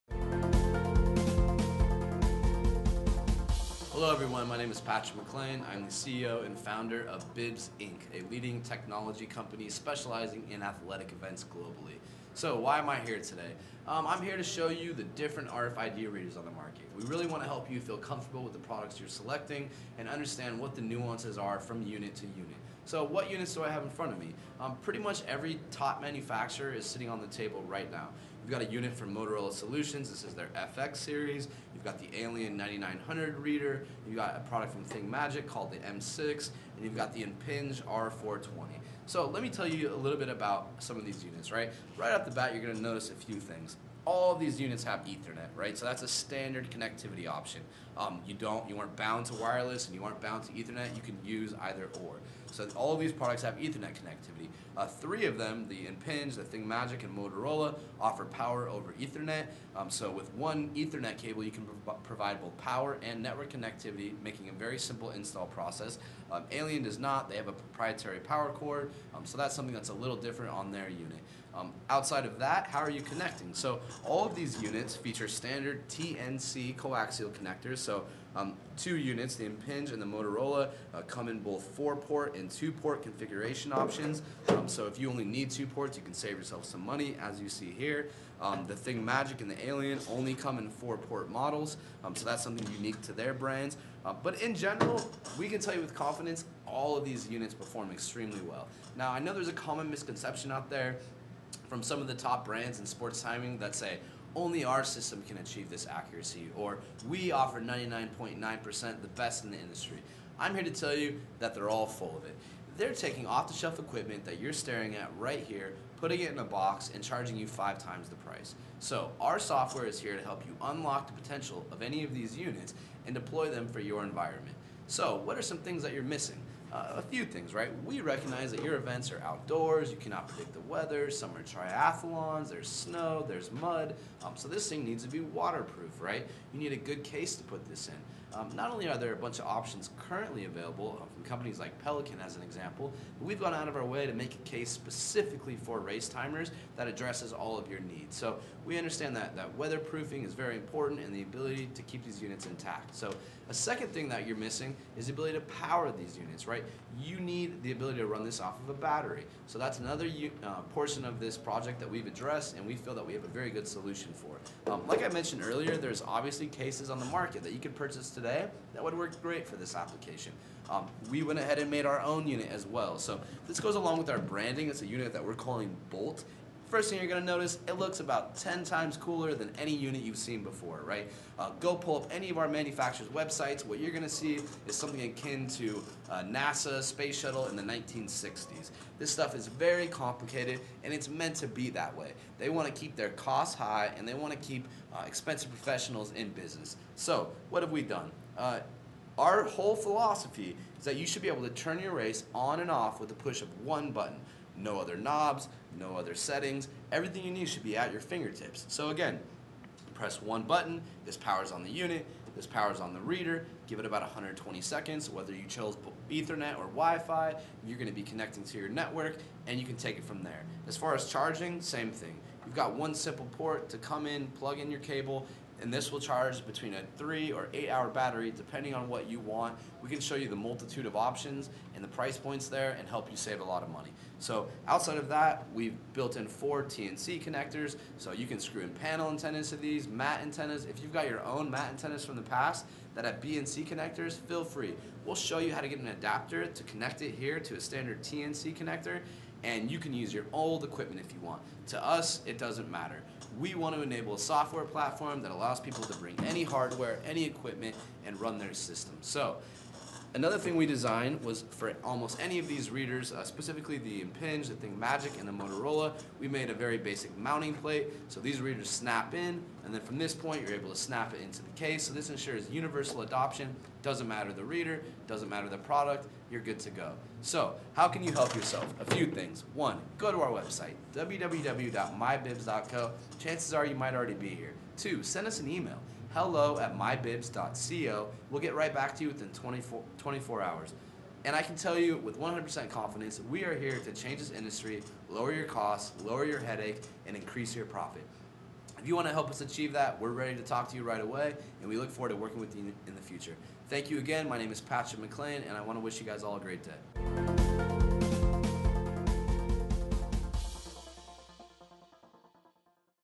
Bolt RFID Reader Explanation
A brief explanation of the various RFID readers on the market today.